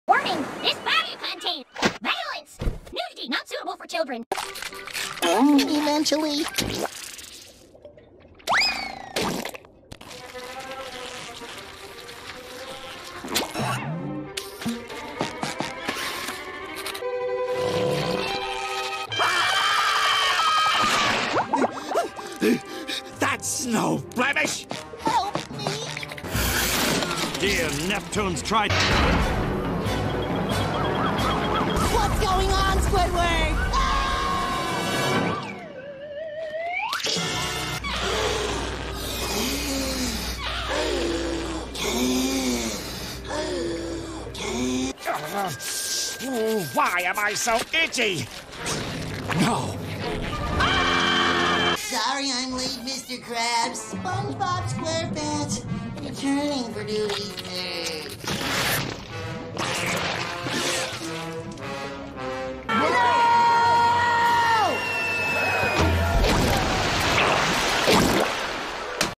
Corrupted Alphabet Lore vs Spongebob
If the Darkness Took Over Alphabet Lore vs Spongebob animation
In this video, I made an animation of how spongebob gets infected vs alphabet lore animation